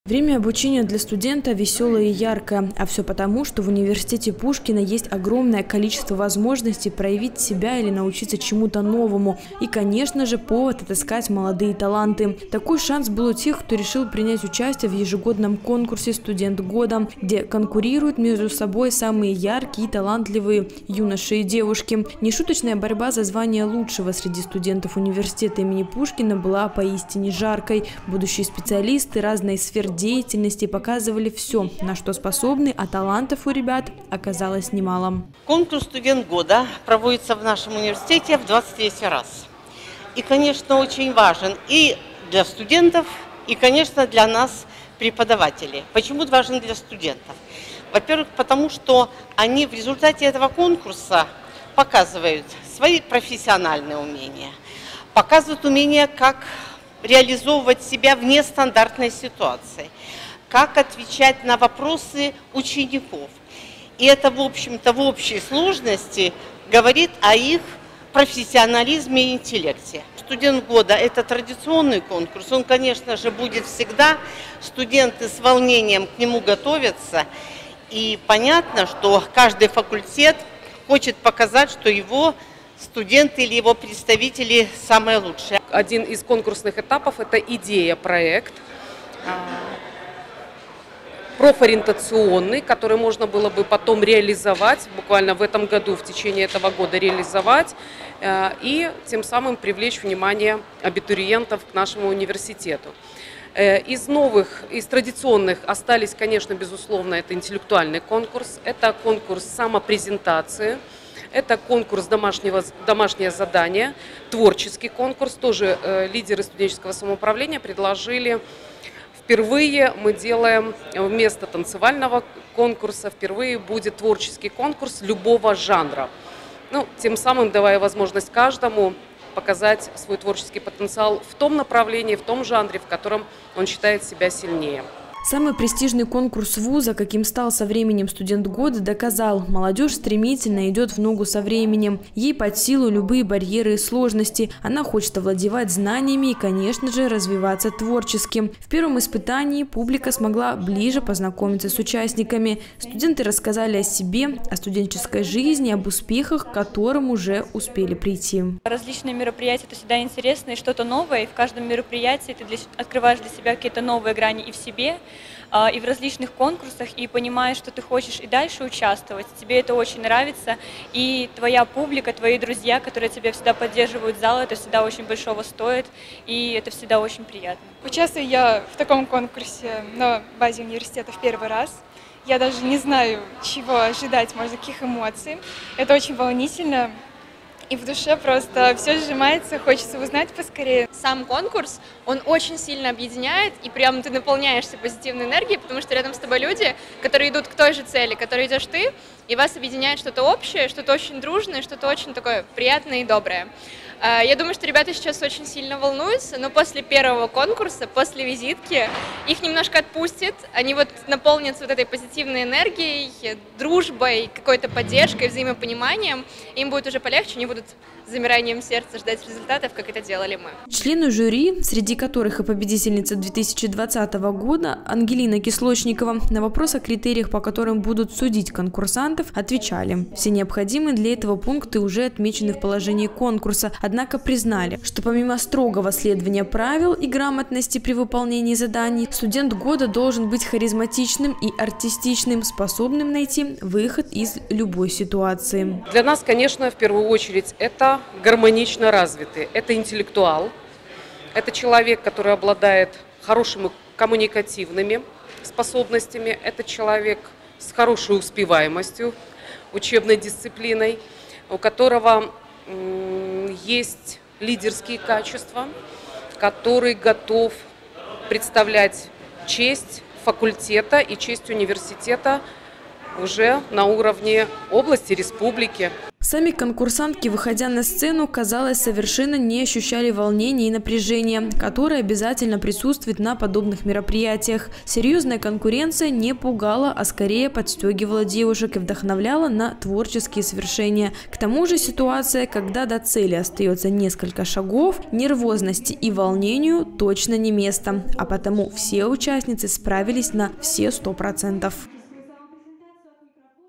2022-12-22 г. Брест. «Студент года – 2022» в УО «БрГУ им. А.С. Пушкина». Новости на Буг-ТВ.
Студенчество –это не только постоянные лекции, экзамены и зачёты. Данный факт может подтвердить практически каждый, кто когда-либо принадлежал к самой яркой, активной и прогрессивной части общества. Так каждый год студенты Брестского государственного университета им. А.С. Пушкина с особым волнением и одновременно радостью ожидают традиционное мероприятие под названием «Студент года». Конкурс среди учащихся вуза, проявивших себя в разных областях и прошедших ряд серьёзных предварительных испытаний – в этом году был не менее долгожданным. 23-е по счёту мероприятие носило название «Поверь в себя». В состав участников вошли талантливые, одарённые, и интеллектуально подкованные студенты. За высокий титул боролись восемь представительниц разных факультетов.